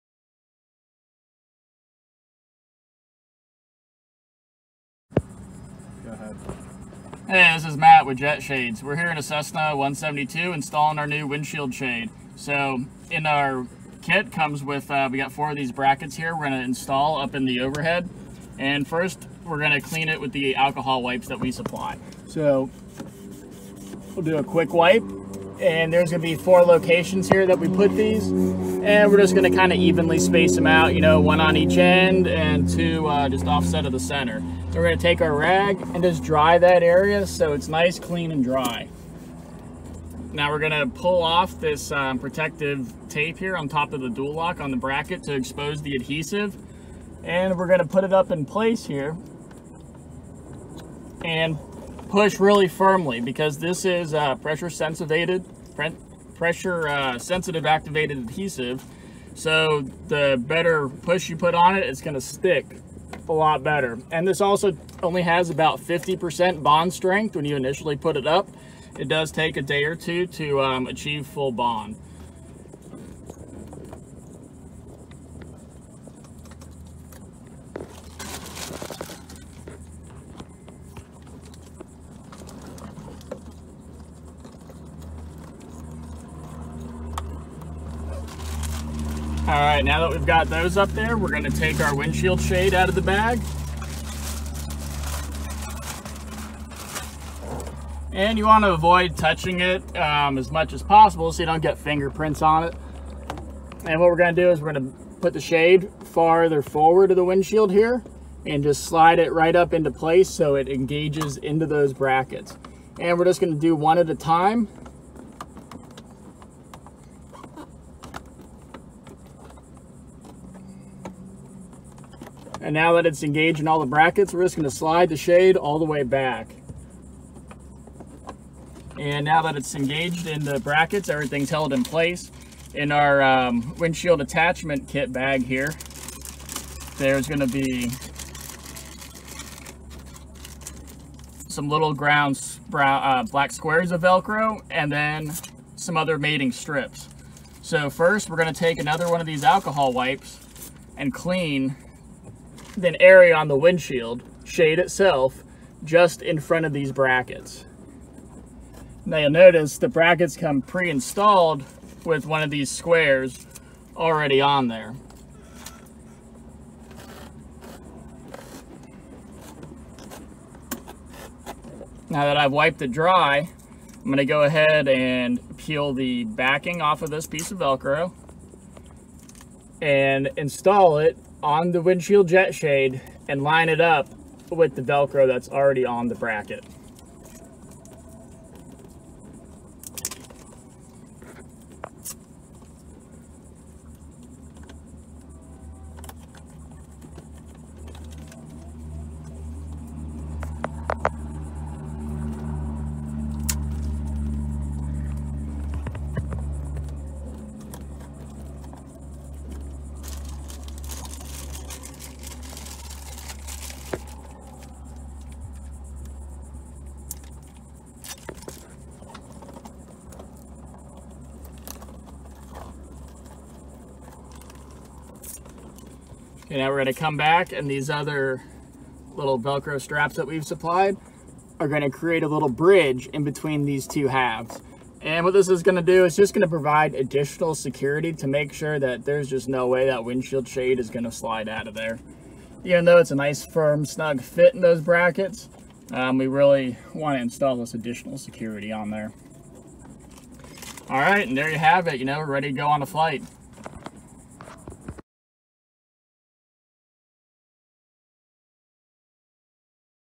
Jet Shades install for Cessna 172
Windshield Jet Shades to block heat and glare in the Cessna 172.  Easy install, no STC needed, no tools needed.  Shade is removable at any time through the use of the brackets provided which attaches to the metal windshield frame by hook and loop fasteners.